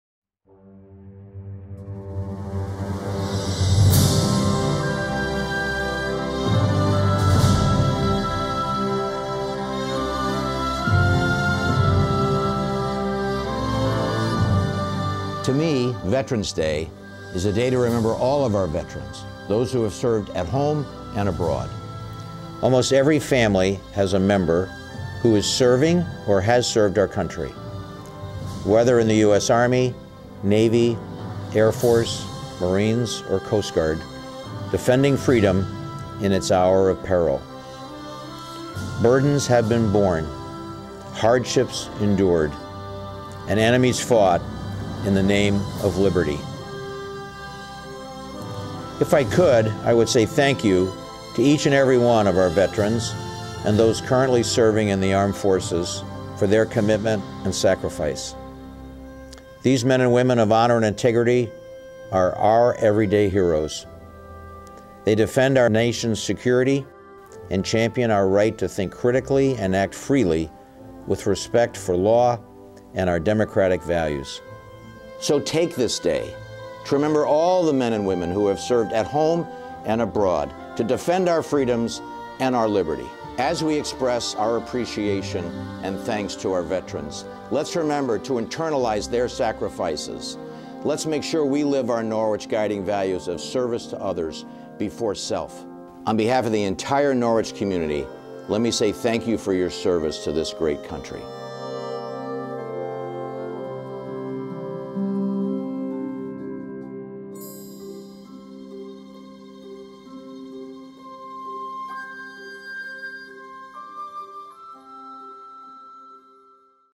Norwich University Honoring Those Who Serve Our Nation on Veterans Day 2016
A special Veterans Day message for 2016 from Norwich University President, Richard W. Schneider, RADM, USCGR (Ret.)